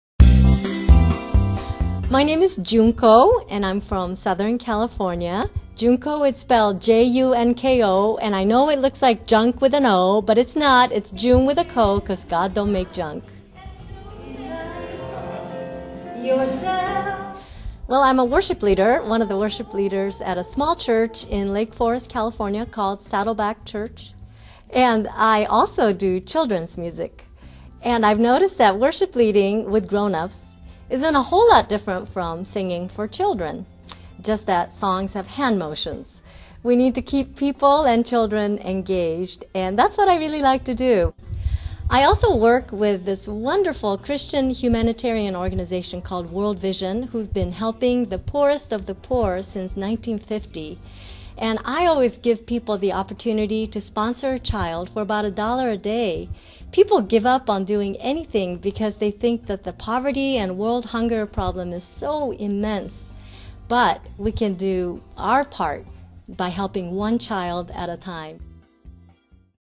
Junko Interview
Interview of Junko for JEMS PNW Spring Concert 2009.